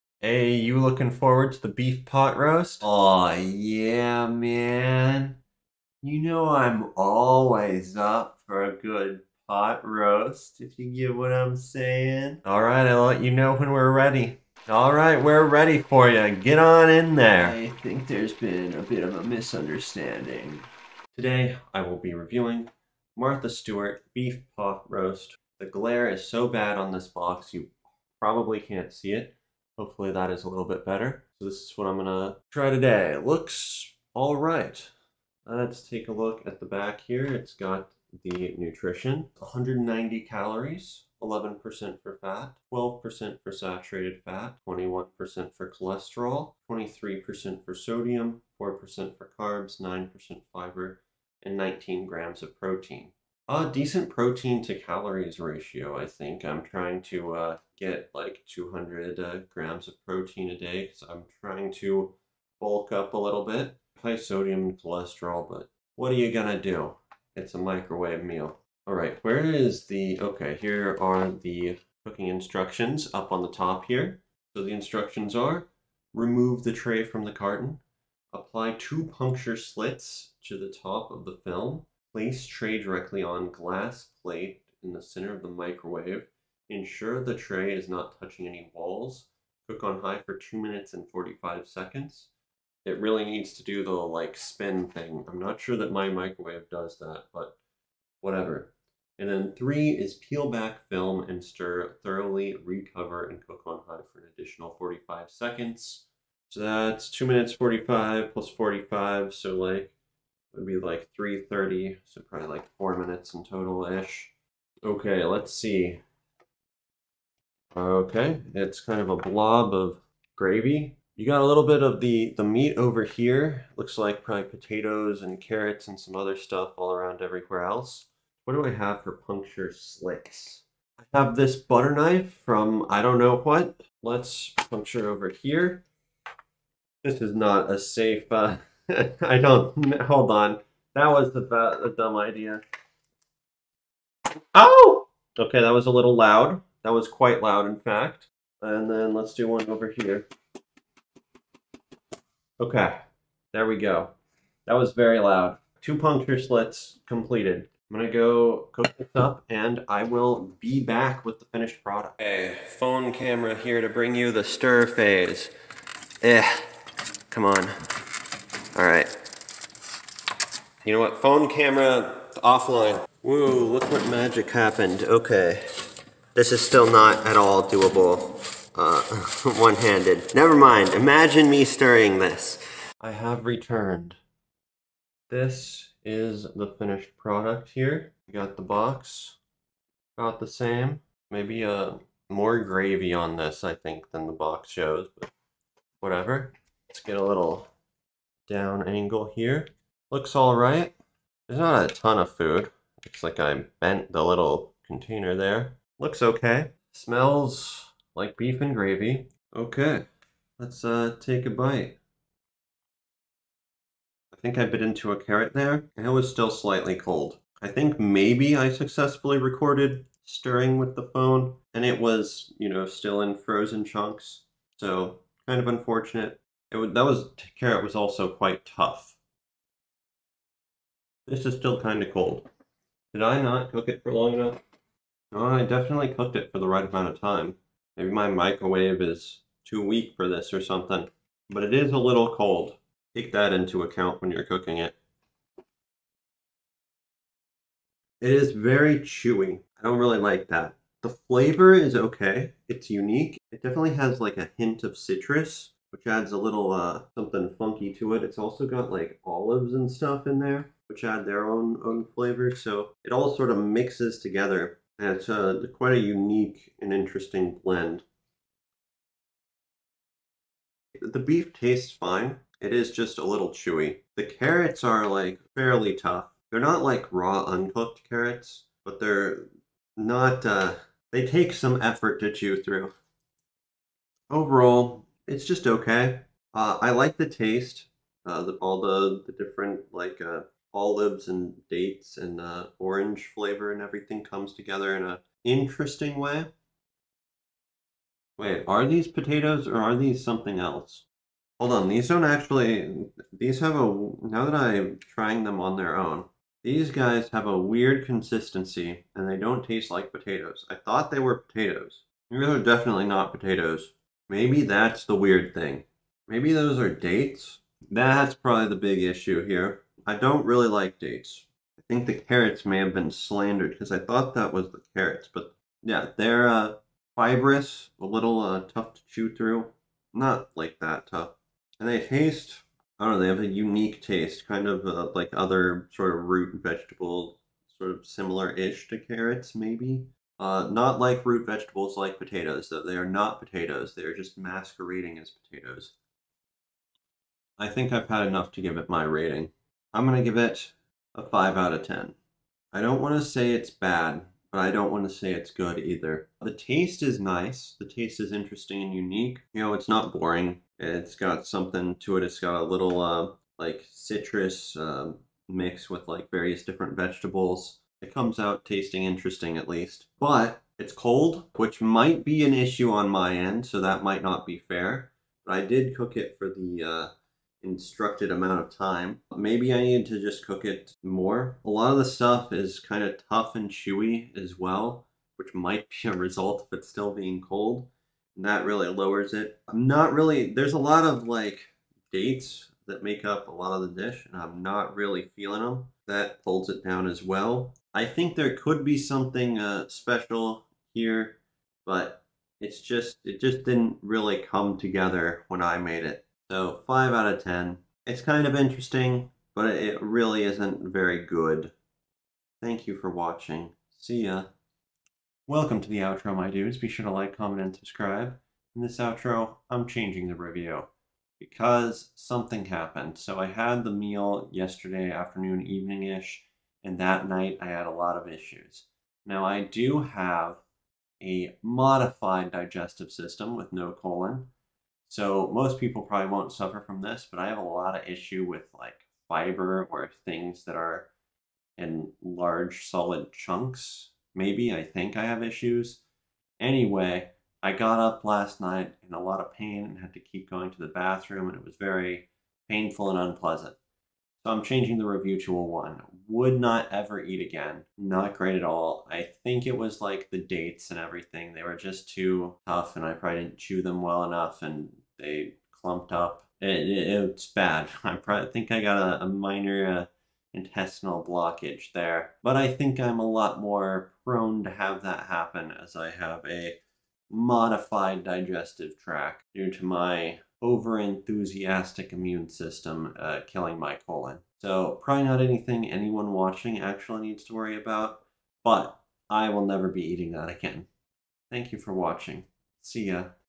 Martha Stewart Microwave Beef Pot Roast Review
I review Martha Stewart Microwave Beef Pot Roast